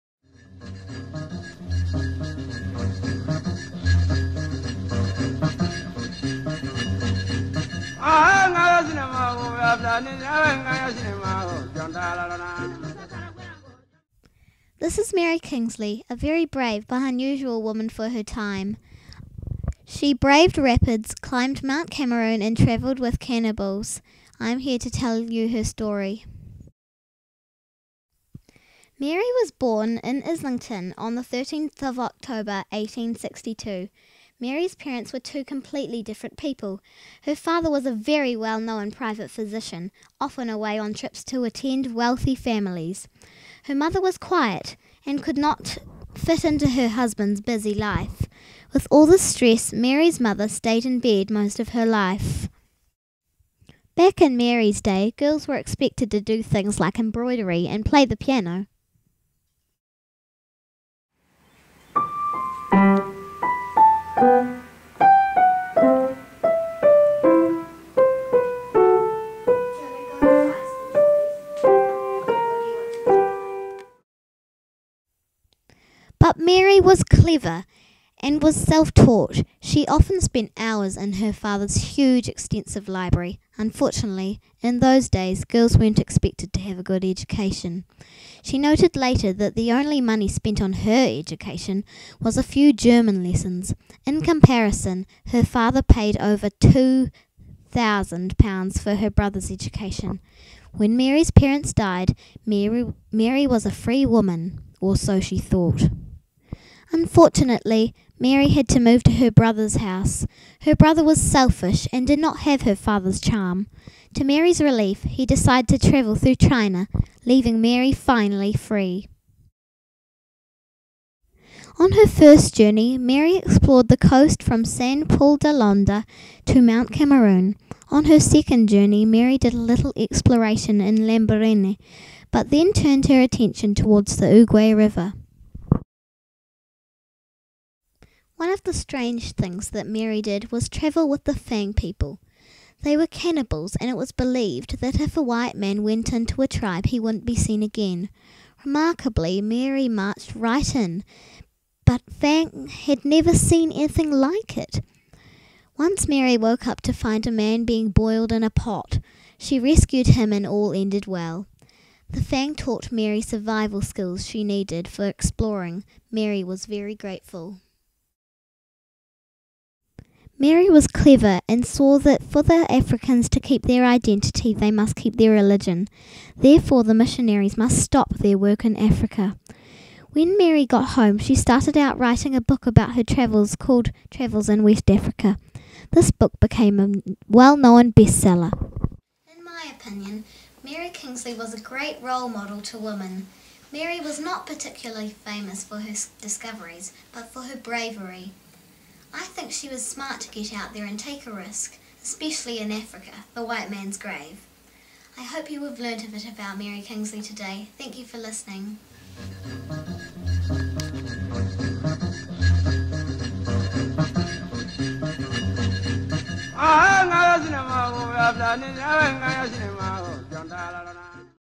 Mary Kinglsey Presentation
A presentation on Mary Kingsley, an English writer and explorer who greatly influenced European ideas about Africa and African people.